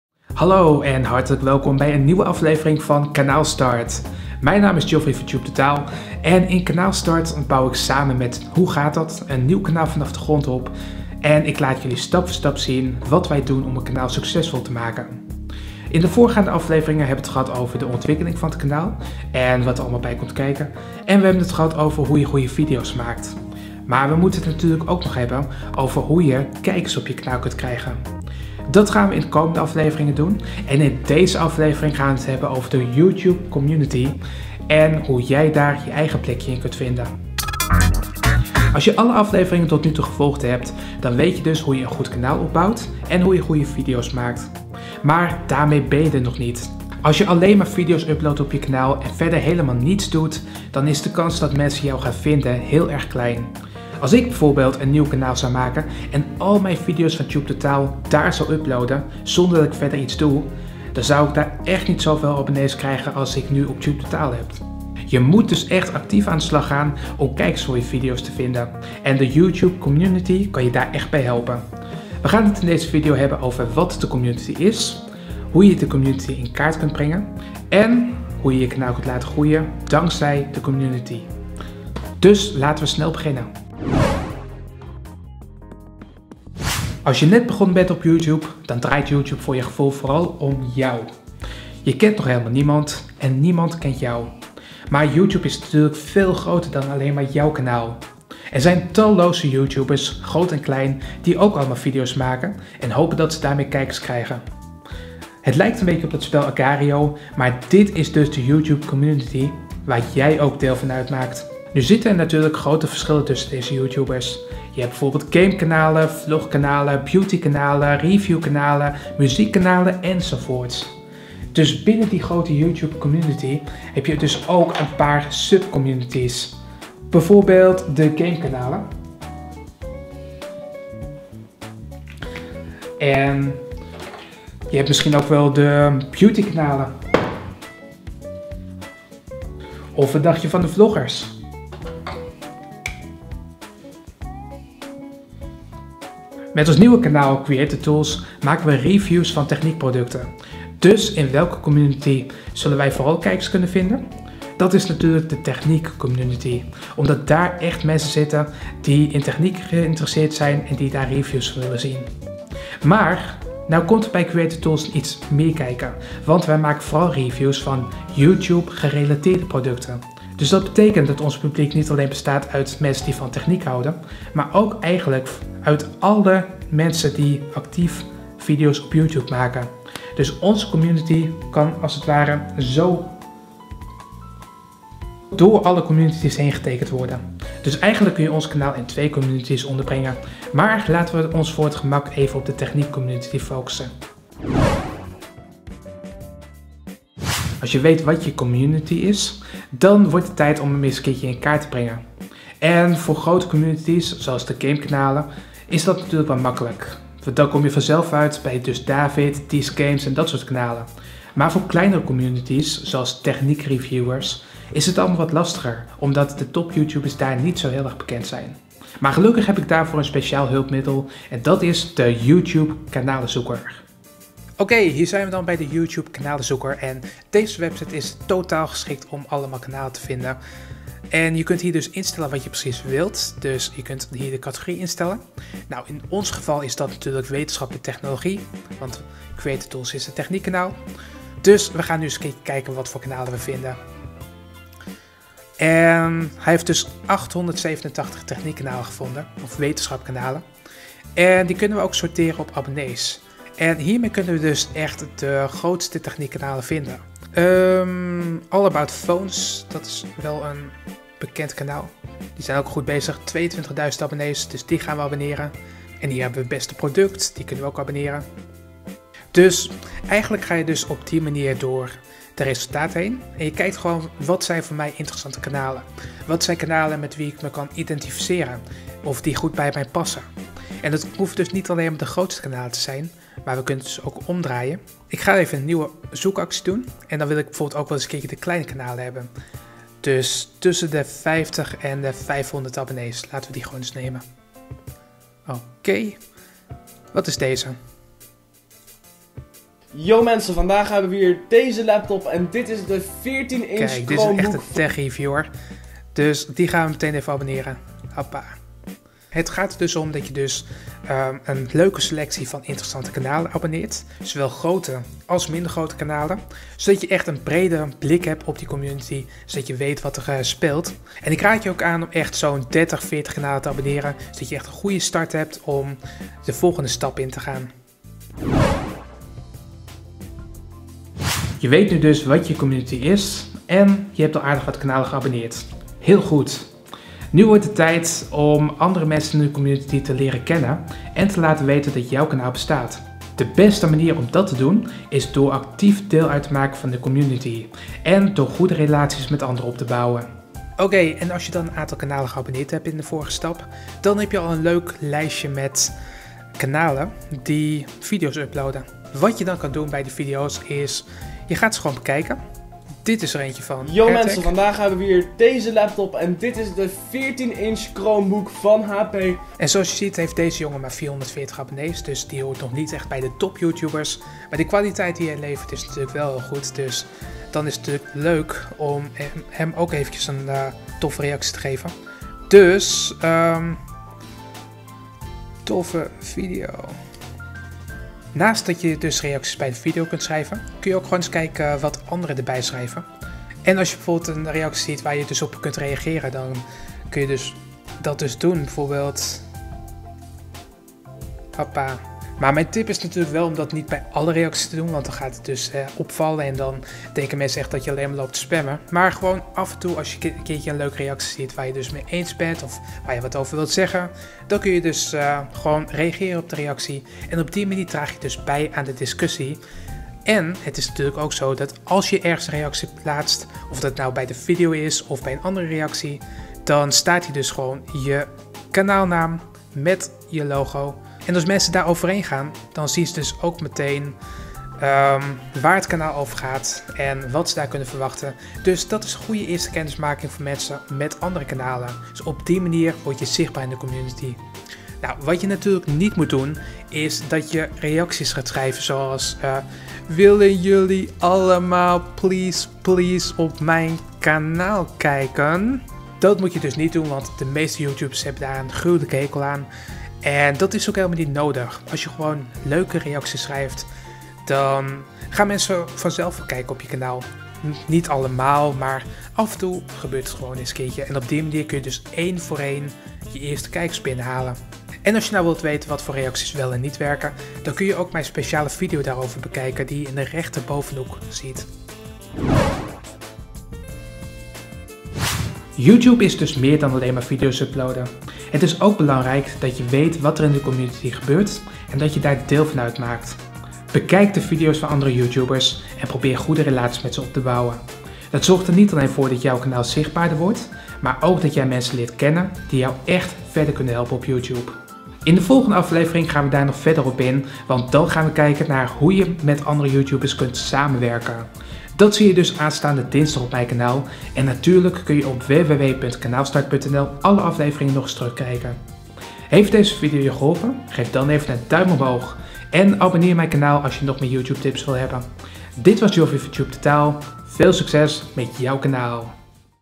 Maak deel uit van de YouTube Community! - KanaalStart #10
YouTube is méér dan alleen maar video's uploaden en hopen dat mensen die toevallig vinden. Als je echt kijkers op je video's wilt krijgen, moet je deel uitmaken van de YouTube Community. In deze video leg ik uit wat ik daarmee bedoel, en hoe je dat voor elkaar krijgt.